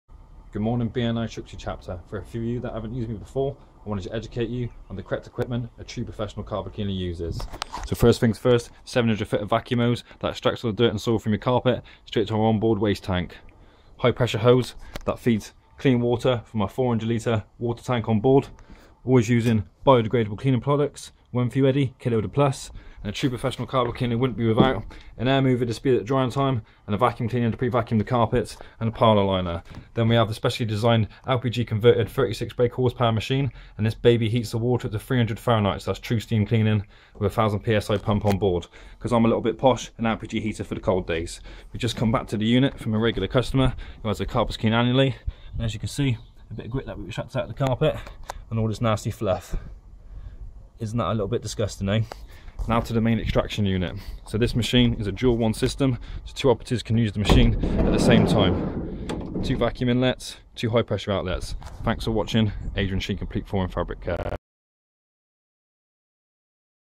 60 seconds BNI (Tewkesbury) Presentation from a Professional Carpet Cleaner in Gloucestershire
With only 60 seconds to fit all this vital information in, I had to show Breakfast Networking International. That there is more to carpet cleaning, than a simple electric machine.

A few business owners chuckled to my video, while getting a loud applause after.
All I can say is it felt good, to show how serious I am. When it comes to restoring soft and hard furnishings. While doing what I do best! And as usual, educating people that there is more to the cleaning and restoration industry, than meets the eye!

P.S. If you could do with some expert advice simply and just ask us a question!